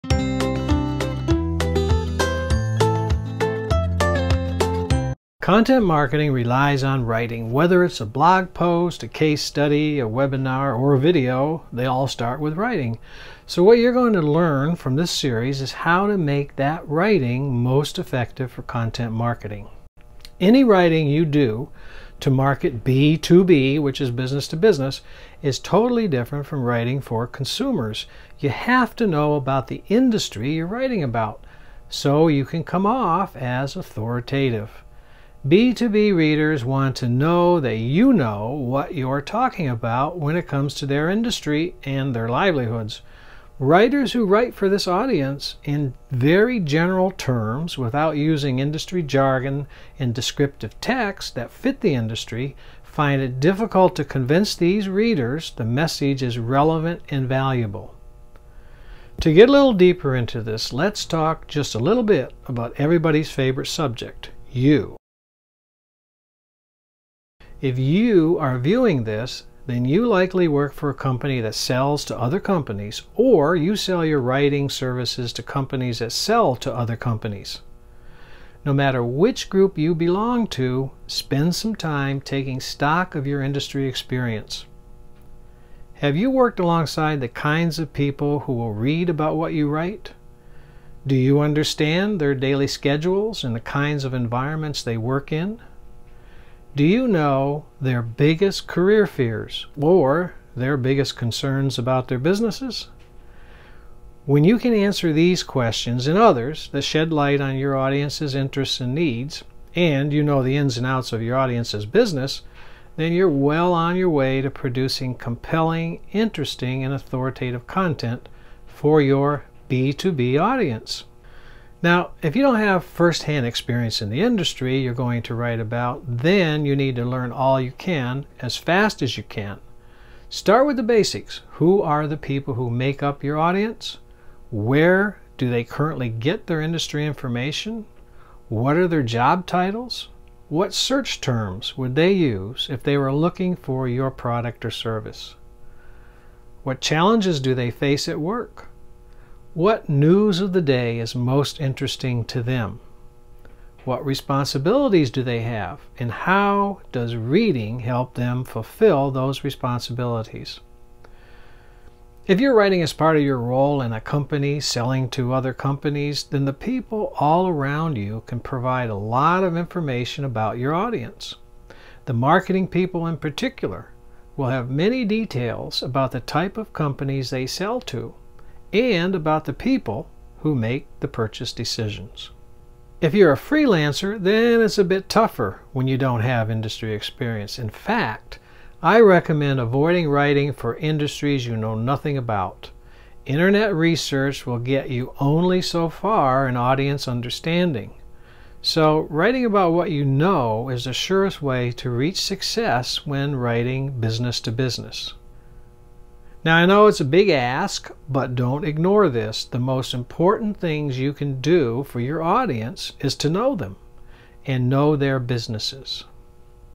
First Steps in Writing for a B2B Audience
These readers want to know that you know their business. What challenges do they face? What makes them get up in the morning and what keeps them up at night? In this short video I leave you with some tips for getting started writing for the B2B world.